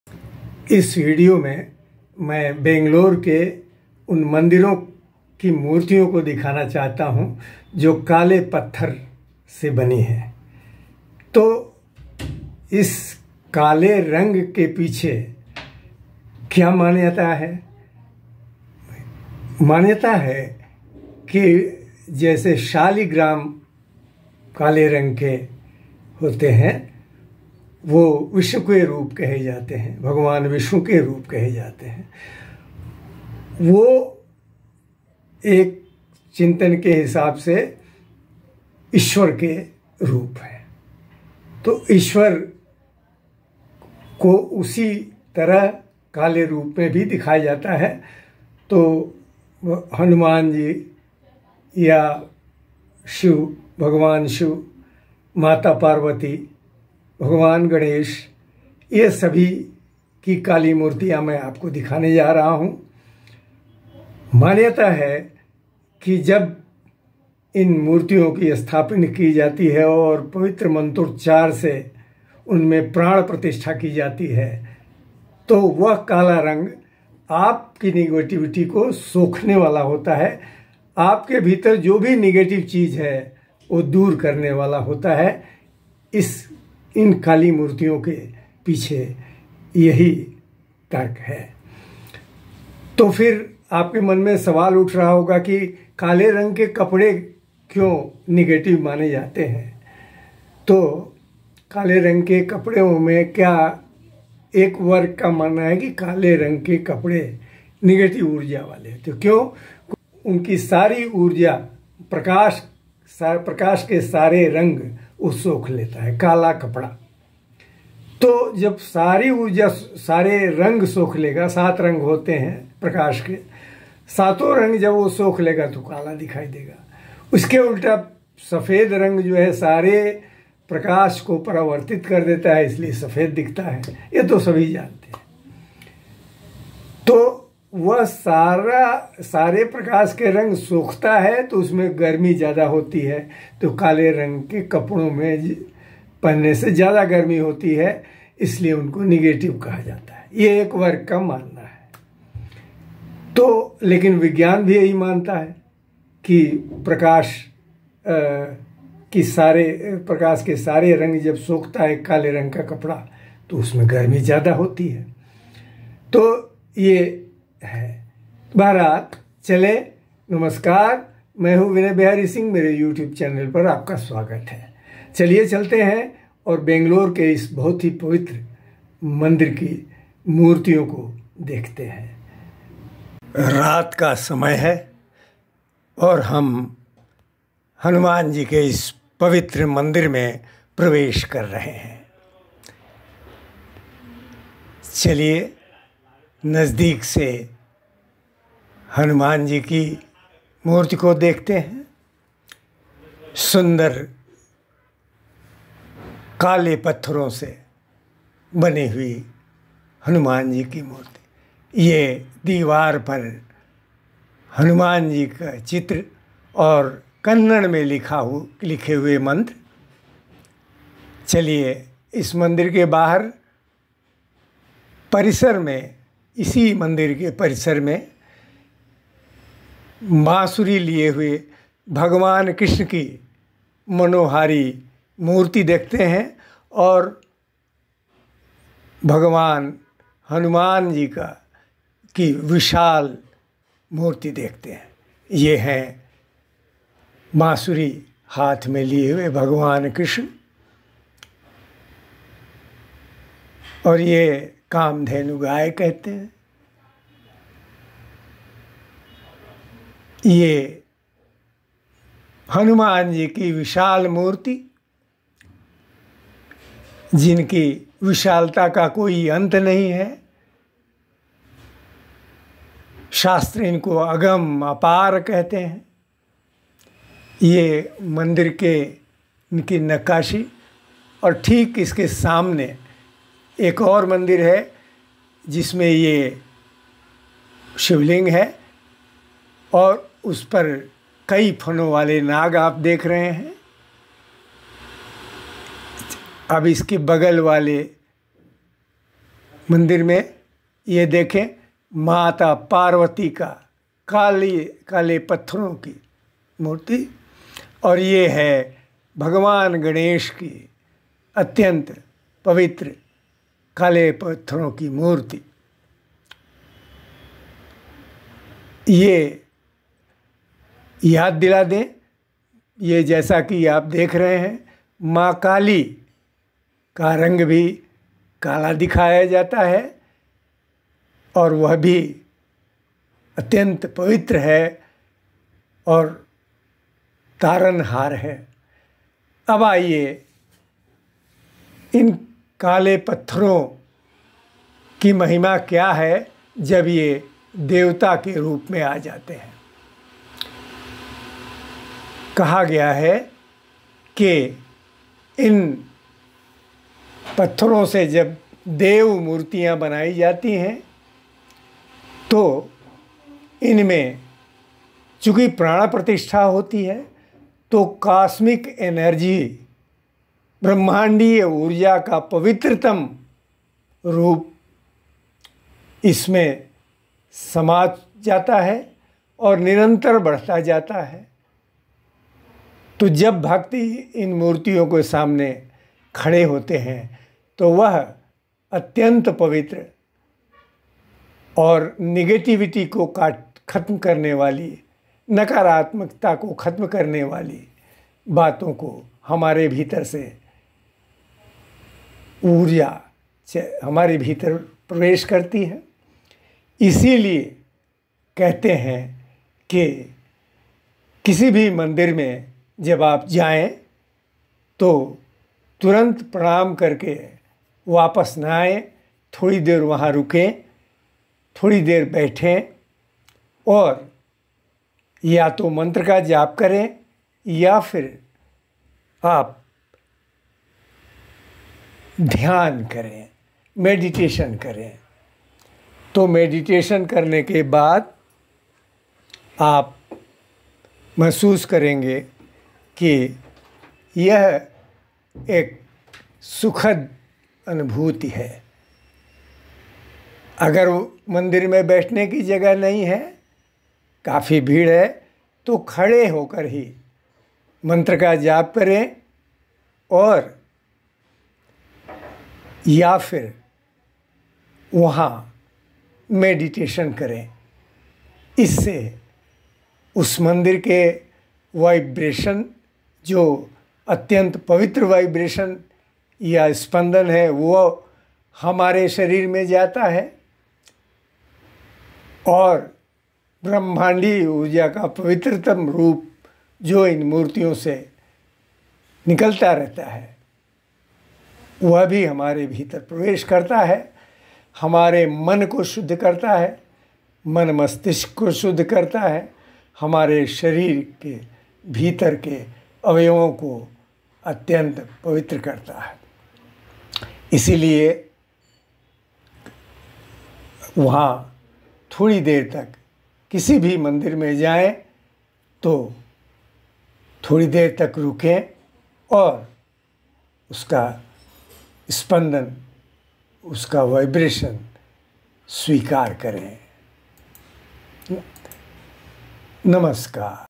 बंगलुरु का एक मंदिरः काले पत्थर से बनी मूर्ति का रहस्य
बंगलुरु के मारगोंडनहल्ली स्थित मेन रोड पर एक मंदिर की हनुमान जी और अन्य देवताओं की काली मूर्ति पर केंद्रित यह वीडियो आपको बहुत सी जानकारियां देगा।